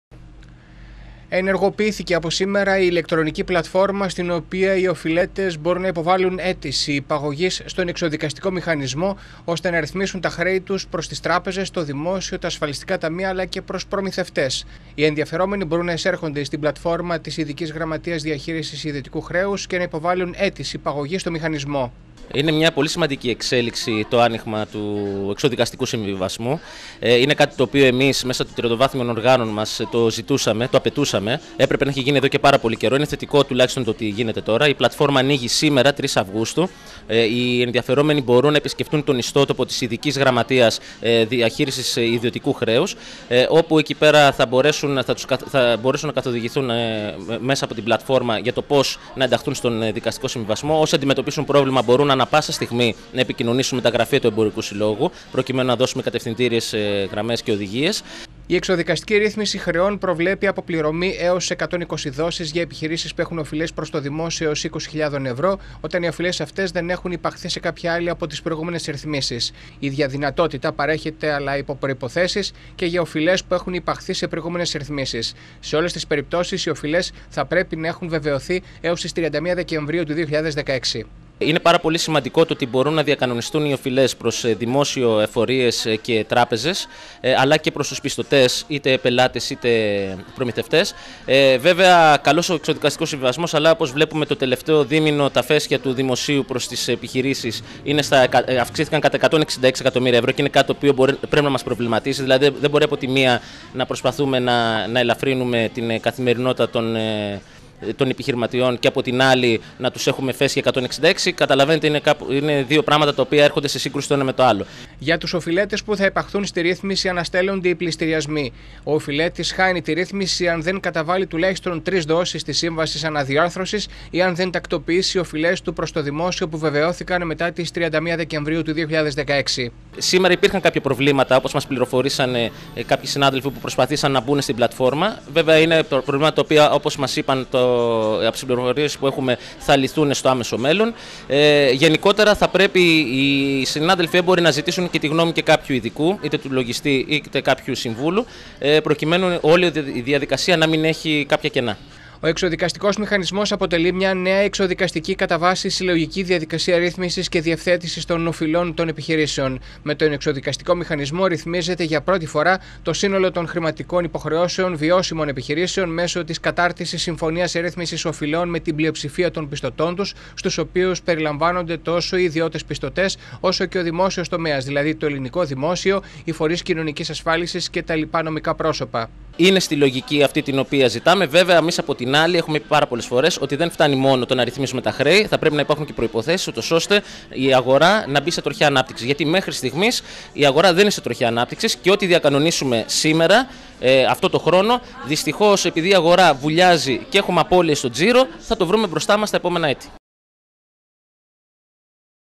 Βόλος Εκκίνηση για τον εξωδικαστικό μηχανισμό 030817
Αρκετές Βολιώτικες επιχειρήσεις έκαναν αίτηση μέσα στις πρώτες ώρες λειτουργίας της ηλεκτρονικής πλατφόρμας του εξωδικαστικού μηχανισμού για τη ρύθμιση οφειλών των επιχειρήσεων, σύμφωνα με τα πρώτα στοιχεία. Οι ενδιαφερόμενοι μπορούν να εισέρχονται στην πλατφόρμα μέσω του link που υπάρχει στον διαδικτυακό χώρο της Ειδικής Γραμματείας Διαχείρισης Ιδιωτικού Χρέους και να υποβάλλουν αίτηση υπαγωγής στον Μηχανισμό. Στην συνέχεια, η υπόθεση ανατίθεται σε συντονιστή, ο οποίος θα επικοινωνήσει με τους πιστωτές για λύση σχετικά με τον τρόπο διαχείρισης των χρεών.